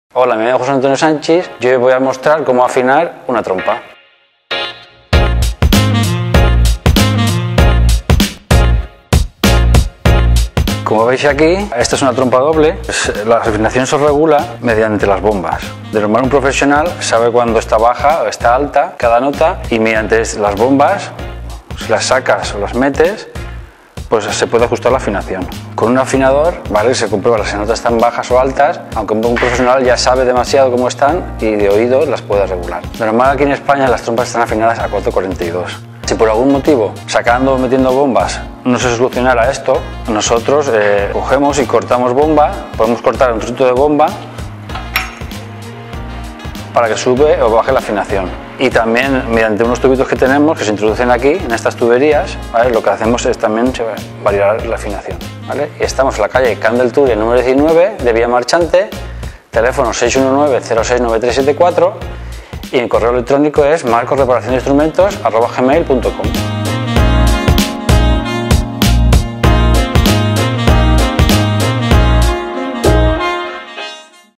Cómo afinar una trompa | Taller Marcos
Afinar una trompa es algo fácil y práctico para quienes ya son músicos experimentados. 👌 ¿Qué ocurre cuando las bombas ya no dan para más?. Hay trucos sencillos que se hacen en talleres como el nuestro.

Taller Marcos. Reparación de Instrumentos Musicales, calle Camp de Túria 19B, Vilamarxant. Valencia. 💻 Página web